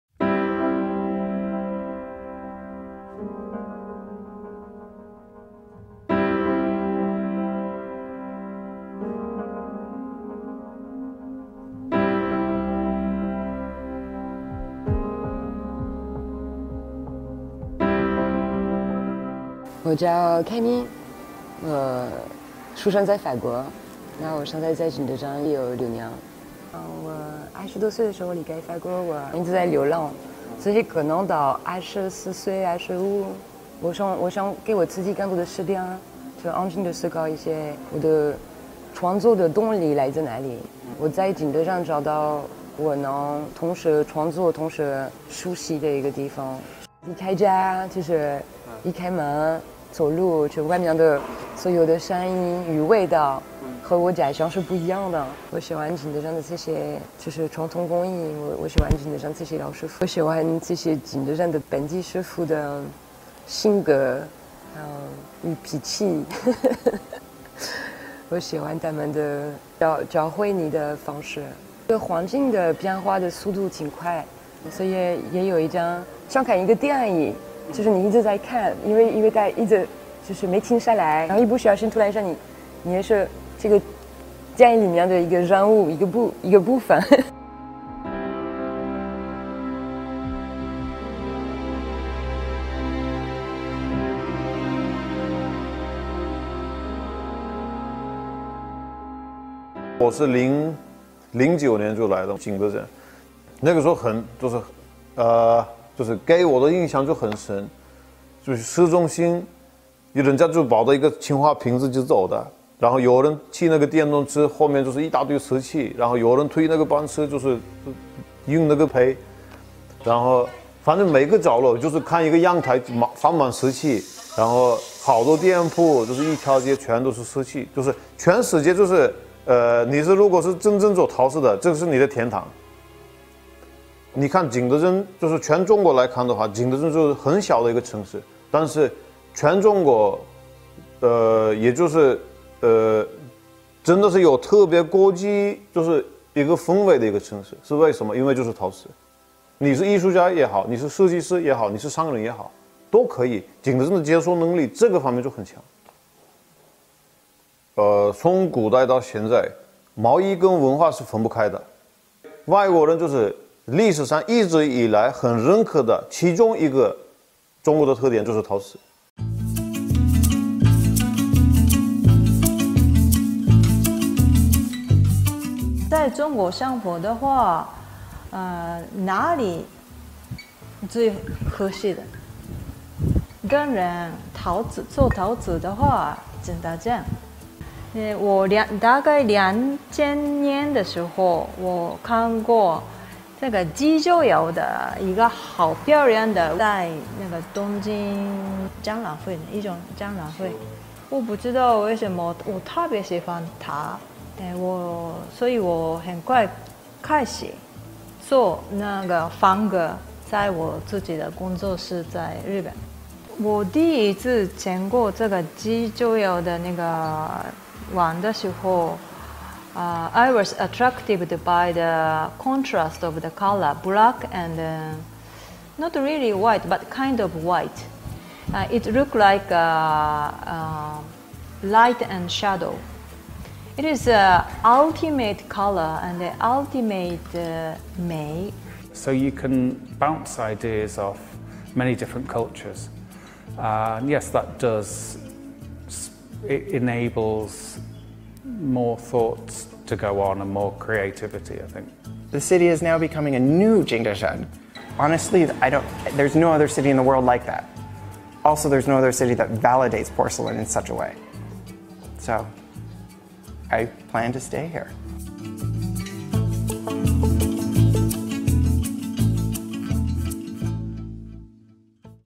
GLOBALink | Foreign potters pursue dreams in China's "porcelain capital"
Dubbed as the "porcelain capital" of China, Jingdezhen, a city with 1,700 years of ceramic-making history in east China's Jiangxi Province, is home to not only excellent Chinese ceramic workers but also foreign potters.